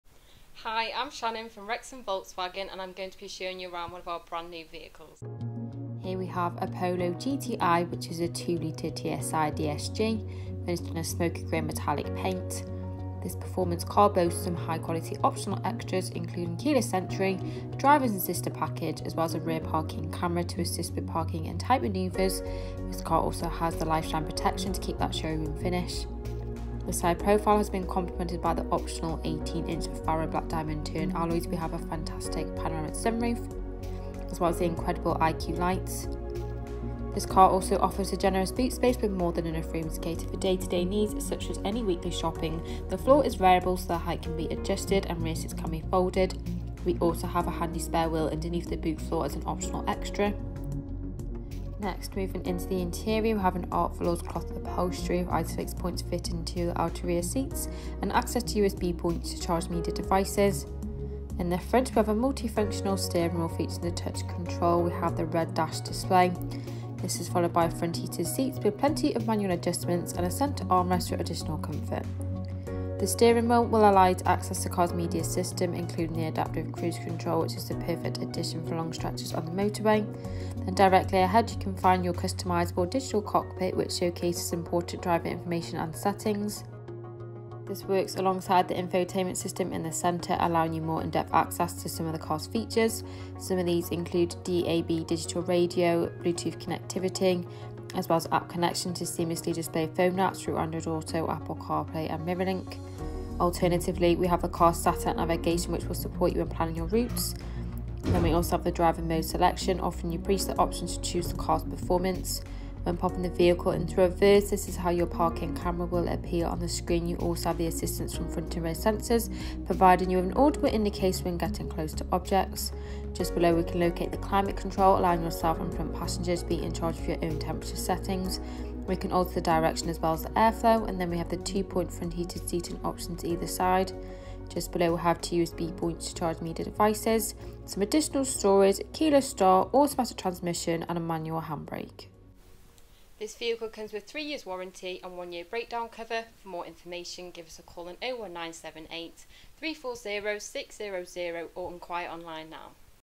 Brand New Volkswagen Polo GTI 2.0 TSI DSG Smokey Grey | Wrexham Volkswagen
Just arrived in stock this fantastic Brand New Volkswagen Polo GTI 2.0 TSI DSG in Smokey Grey metallic paint, is now available for sale from Wrexham VW.

Fuel: Petrol
Transmission: Automatic
Engine size: 2.0L

Optional extras:
Smokey grey metallic paint
18" Faro black diamond turned alloy wheels
Art velours
Rear camera
Space and weight saving spare wheel
Panoramic sunroof
Drivers assistance pack